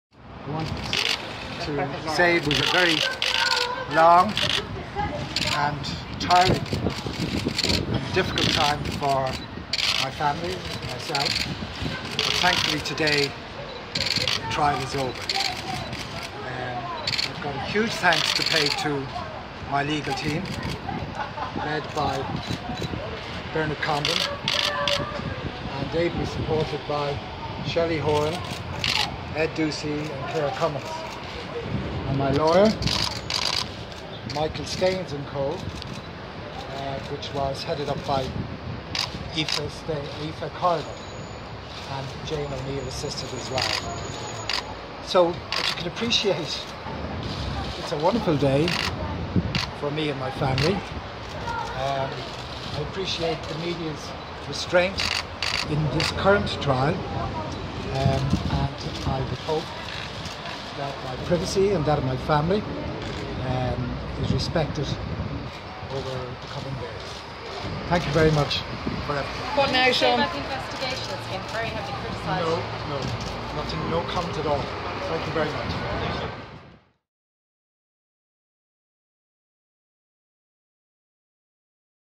WATCH: 'It's a wonderful day' - Sean Fitzpatrick acquitted on all counts
The trial of former Anglo Irish Bank chairman Sean FitzPatrick for allegedly misleading the bank's auditors about millions of euro in loans between 2002 and 2007 has collapsed.